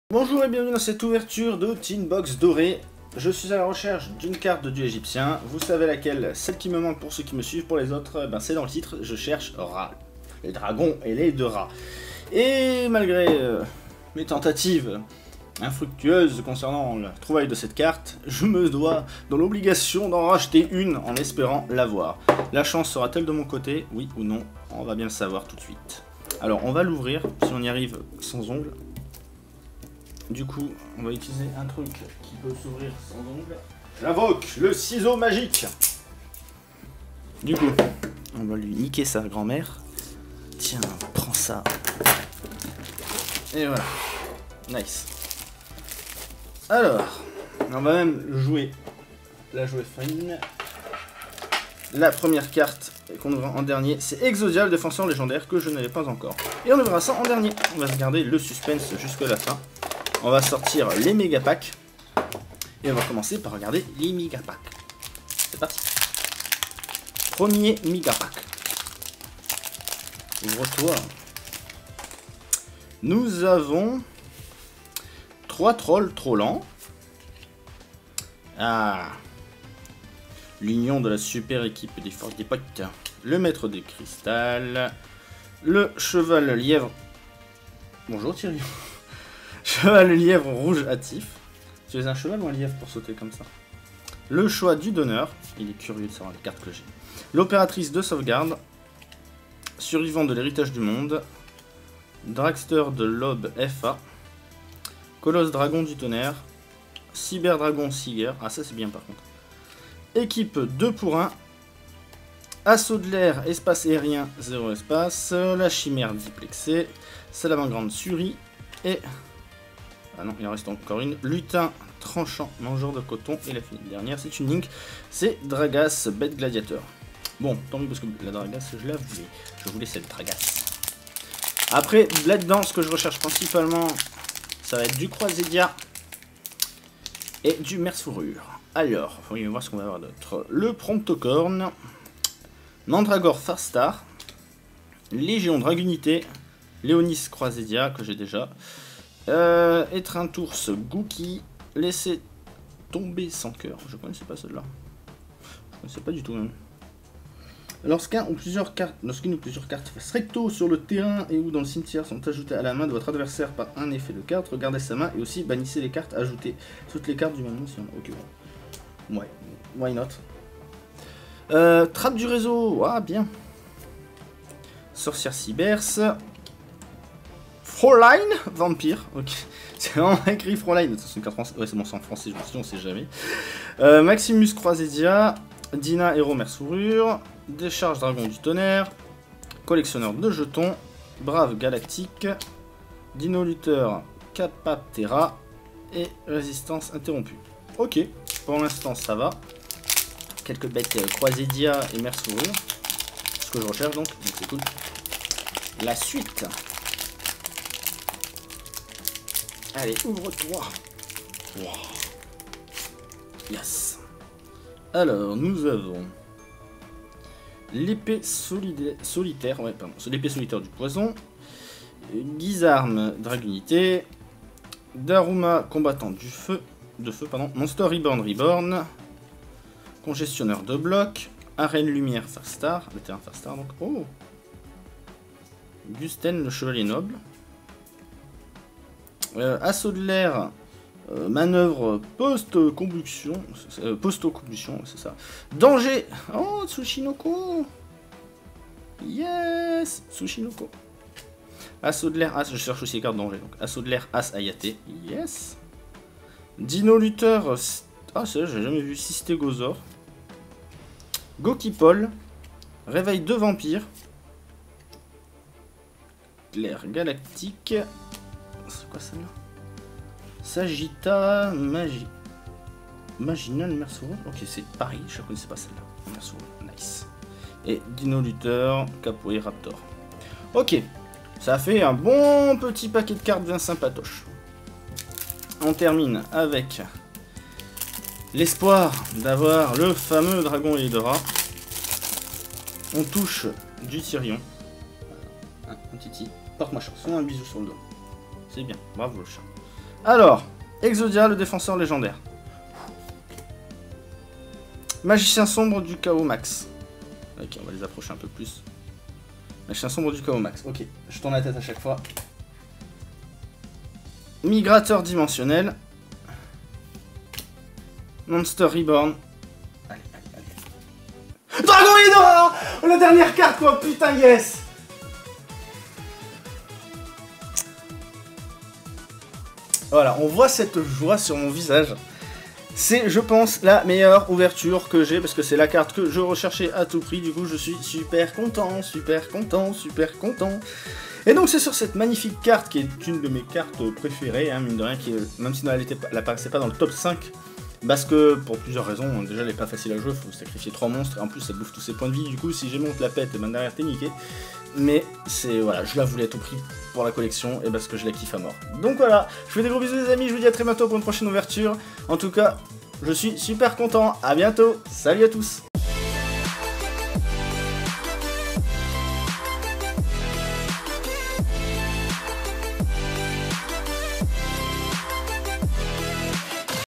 À LA RECHERCHE DE RÂ ! - Ouverture Sarcophage Doré
On recherche la carte de dieu égyptien qu'il me manque !